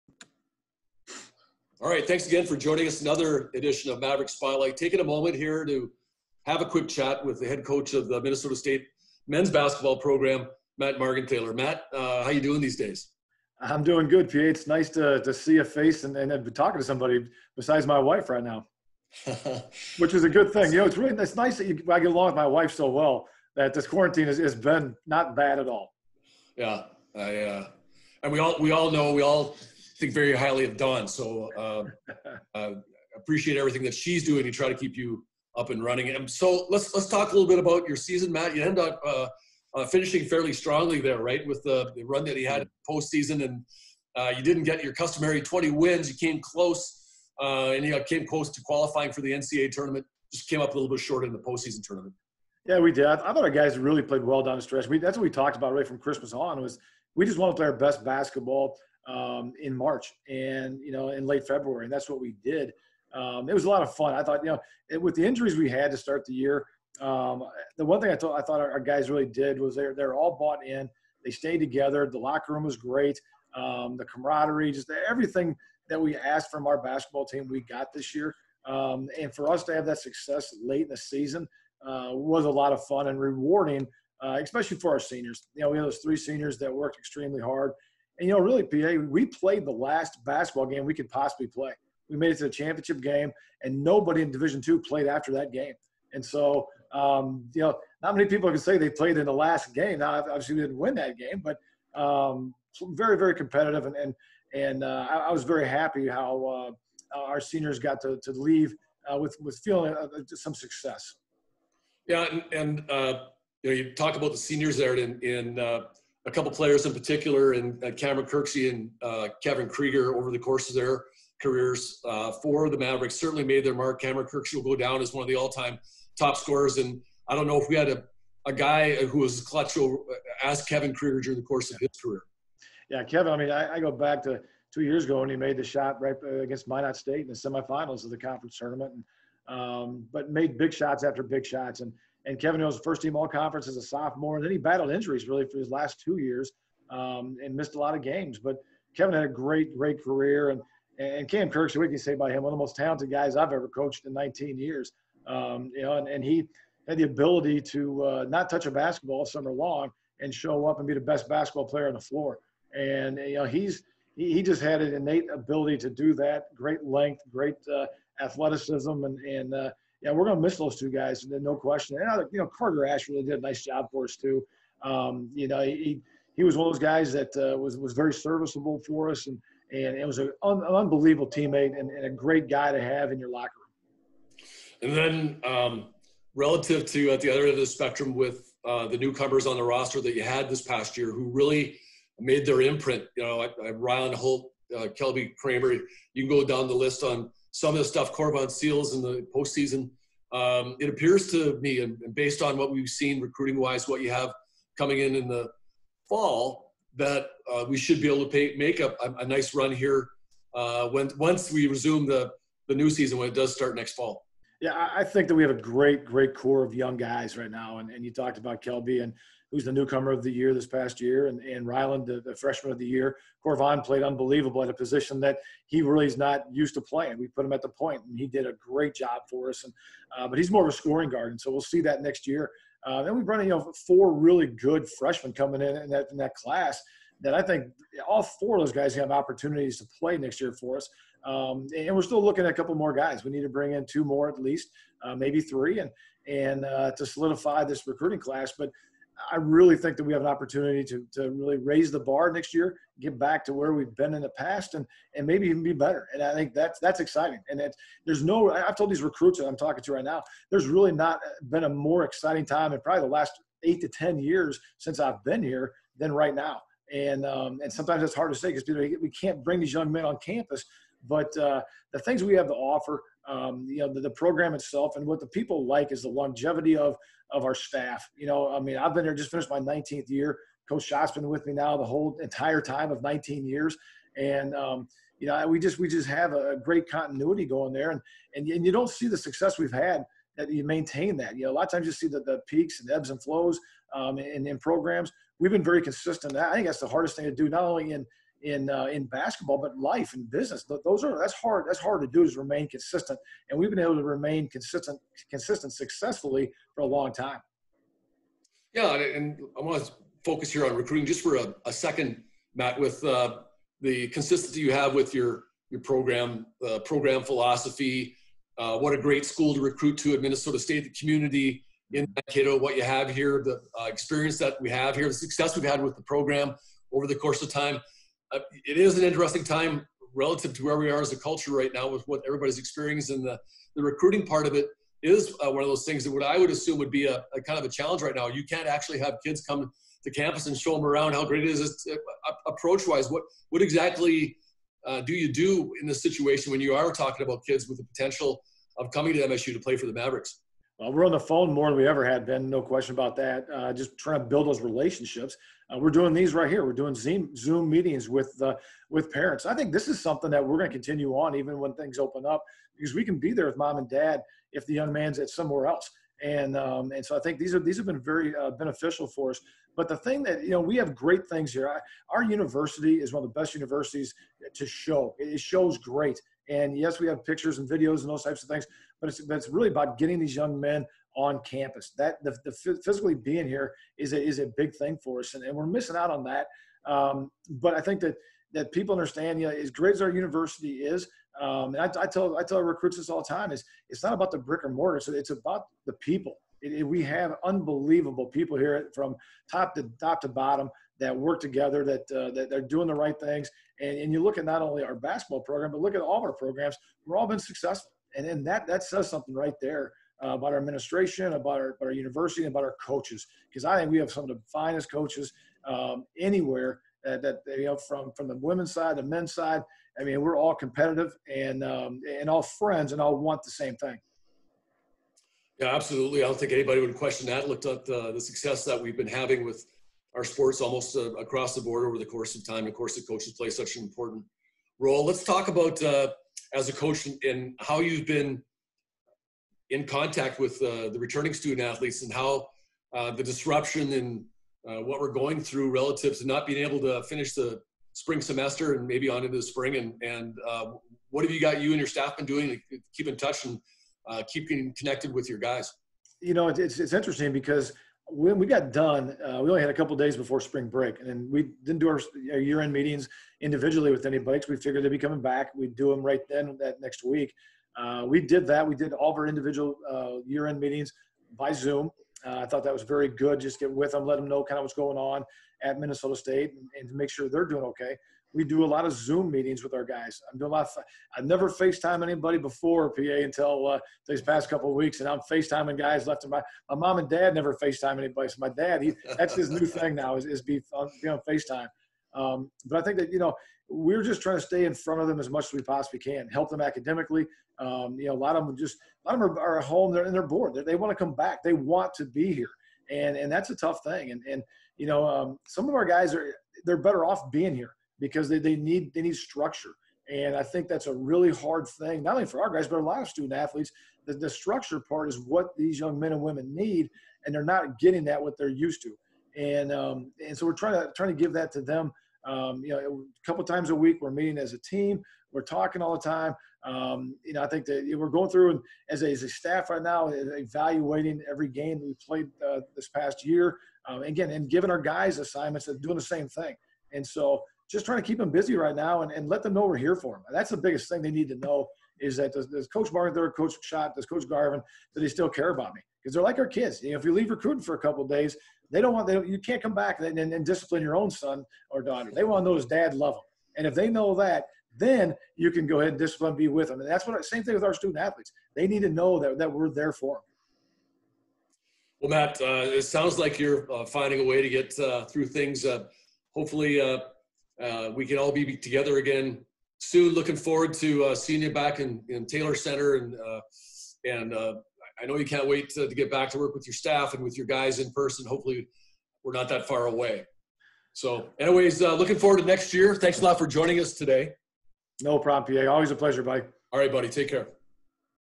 Maverick Spotlight | Matt Margenthaler
Head coach of the Minnesota State men's basketball head coach Matt Margenthaler.  April 15, 2020.